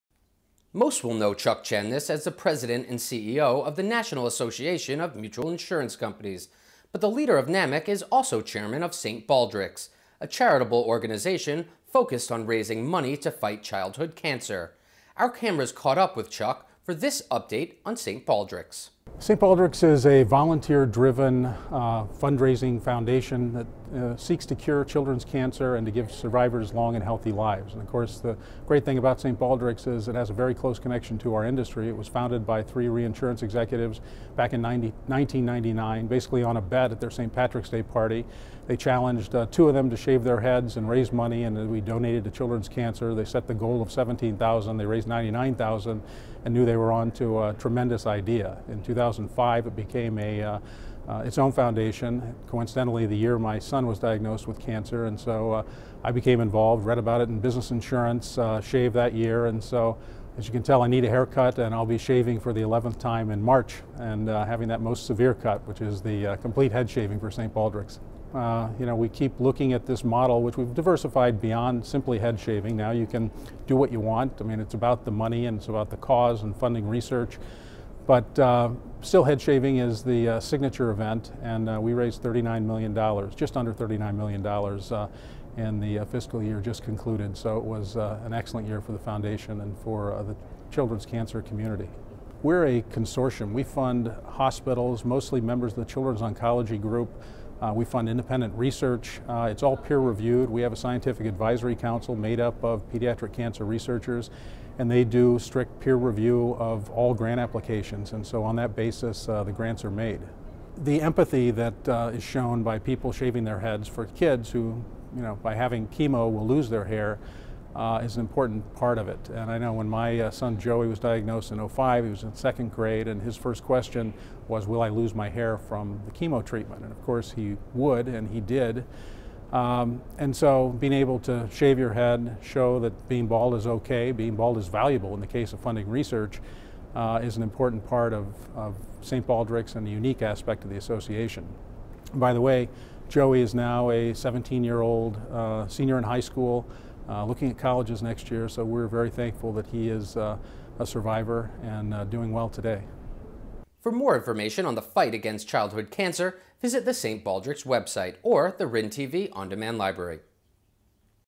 St. Baldrick’s shows kids that being bald is ok, even valuable in funding cancer research
Chuck Chamness is President and CEO of the National Association of Mutual Insurance Companies (NAMIC). , Mr. Chamness is also Chairman of St. Baldrick’s, a volunteer charitable organization focused on raising money to fight childhood cancer. He reminds us that the charity was started by three reinsurance executives at a St. Patrick’s Day Party in 1999. A challenge bet resulted in two shaving their heads for a donation to children’s cancer. While their original goal was to raise $17,000, they actually raised $99,000 and knew they had a “tremendous idea.”

St. Baldrick’s became its own foundation in 2005, the year Mr. Chamness’ son was diagnosed with cancer. He joined the fight that year and had his head shaved for the first time. Mr. Chamness will have his head shaved again in 2015 for the 11th time. He says those who have their heads shaved show empathy for children who will lose their hair as a result of chemotherapy. Mr. Chamness recalls his son being concerned about losing his hair when he had chemo treatments while in second grade. “Shaving your head shows kids that being bald is ok; being bald is valuable in funding research.” Mr. Chamness is happy to report his son Joey is now a 17 years old survivor looking at colleges.

St. Baldrick’s raised nearly $39 million for children’s cancer research in 2014. As a consortium, St. Baldrick’s funds hospitals (most members of the Children’s Oncology Group) and independent research reviewed by a scientific advisory council.

For more information on the fight against childhood cancer, visit the St. Baldrick’s website.